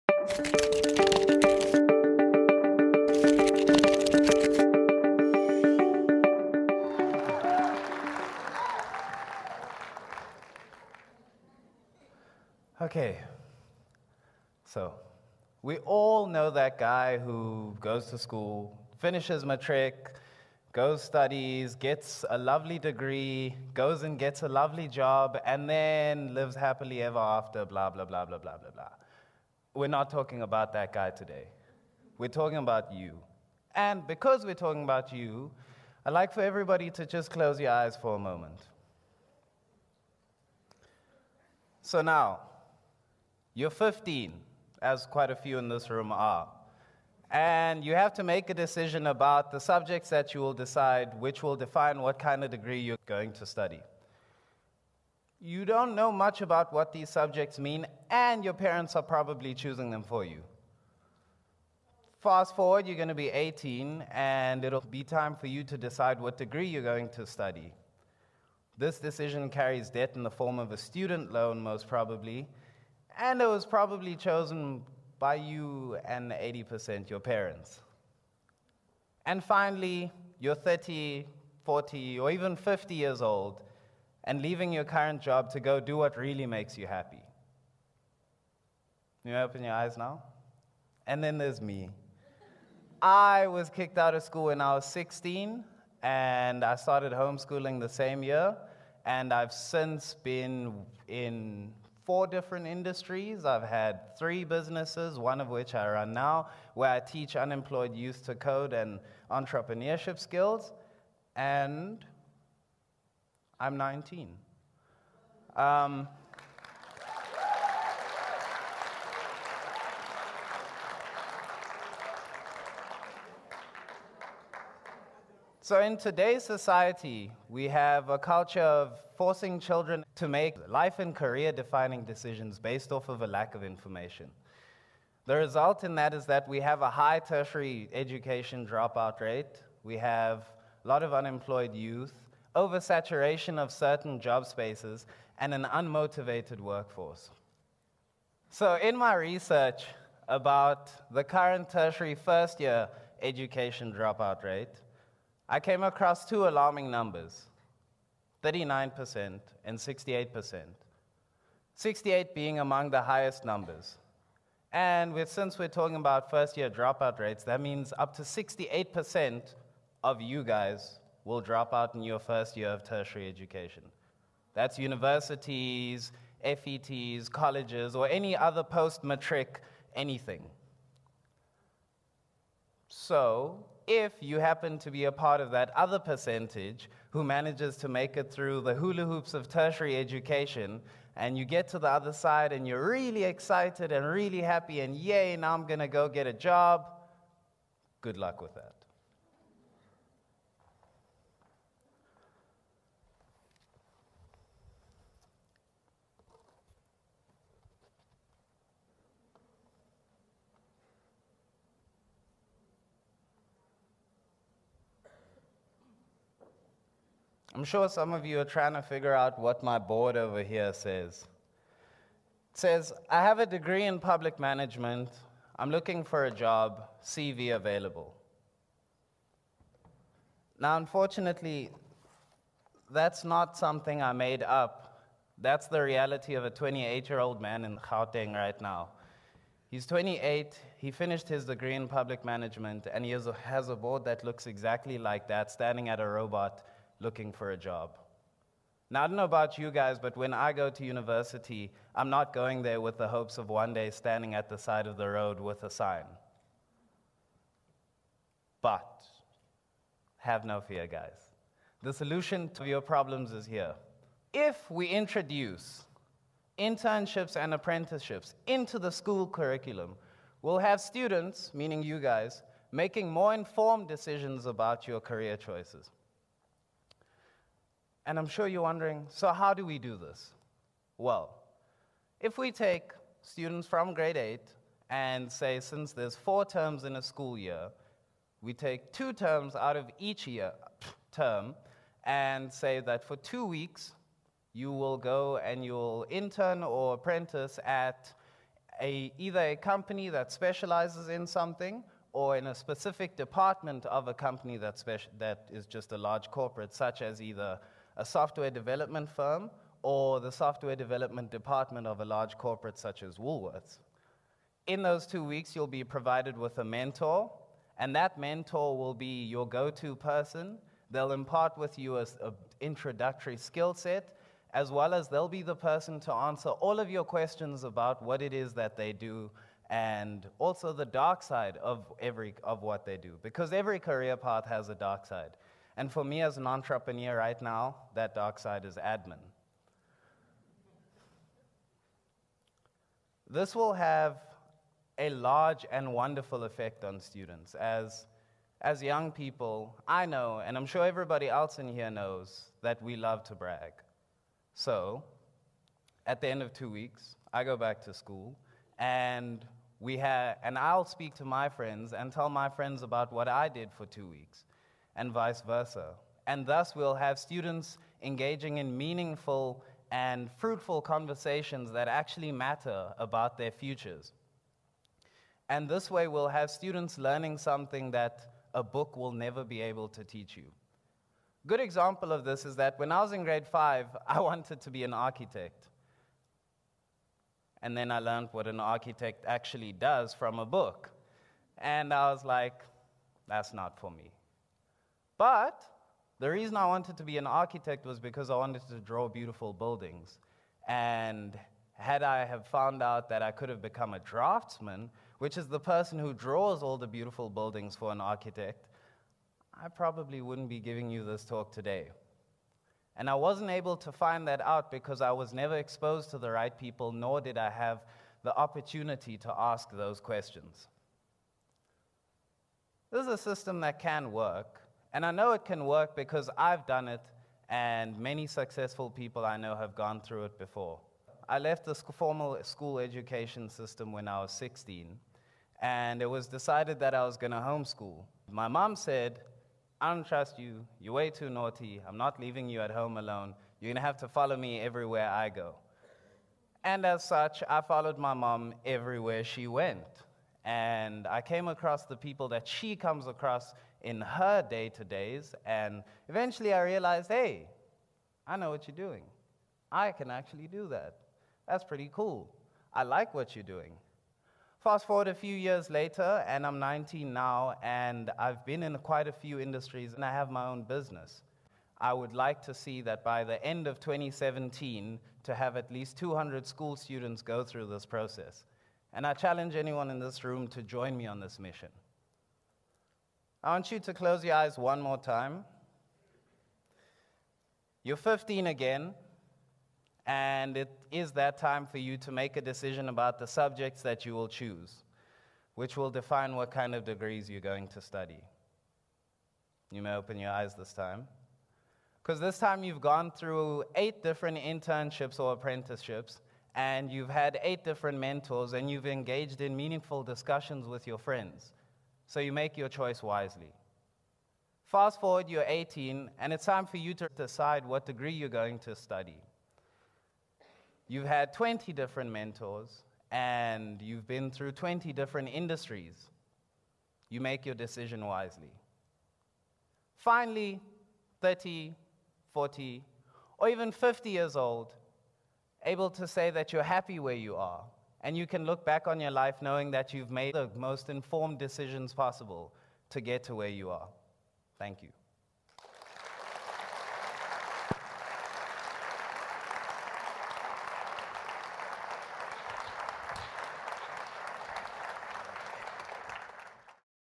Change the way we learn | Lebogang Miya | TEDxYouth@CapeTown
An interesting new option for gaining work experience while at school.

About Lebo:

Lebo is a 19 year old entrepreneur.

After being kicked out of school at 16, he spent a fair deal of time with his mom, observing her line of work.

A few years, many businesses and lots of failures later, Lebo now runs a successful company that teaches youth (18-25) how to code.